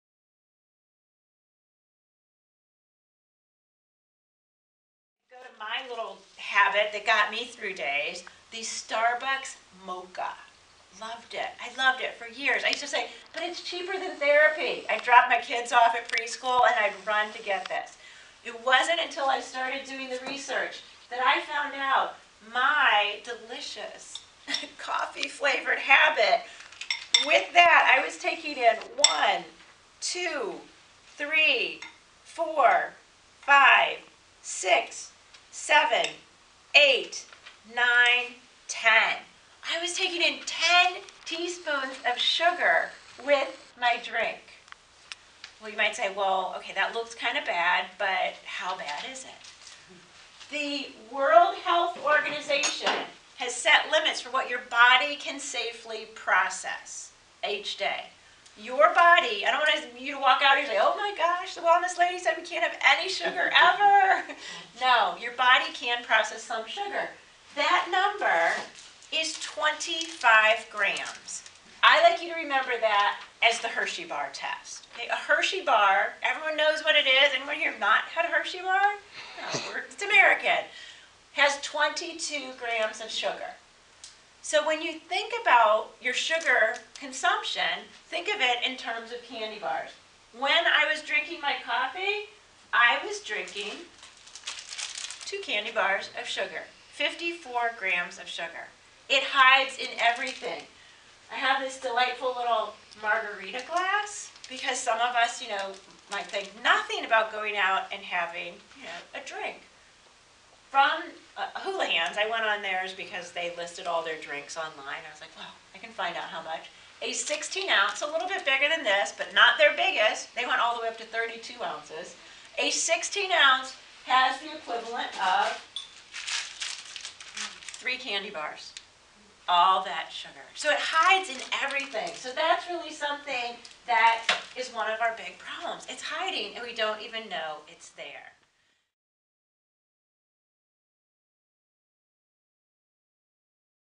Tame Your Sugar Monster: The Candy Bar Rule
Kathy Parry, your real food coach, made a stop at the Morgantown Public Library during her "Tame Your Sugar Monster" program tour on January 19, 2017.  This powerful program revealed the dangers and addictive nature of sugar and how your habit may be aging you and damaging your health.

for more on improving your health through your diet.